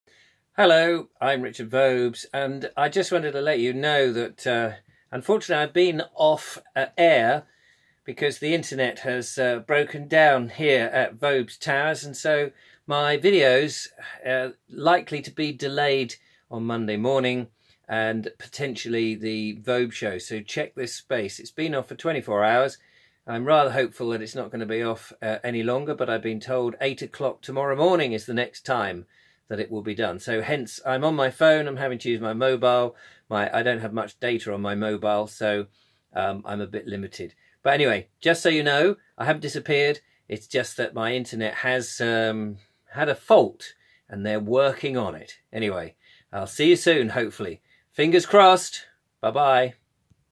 INTERNET BROKEN
A quick message to inform you that my internet connection has a problem and so my videos are a little delayed.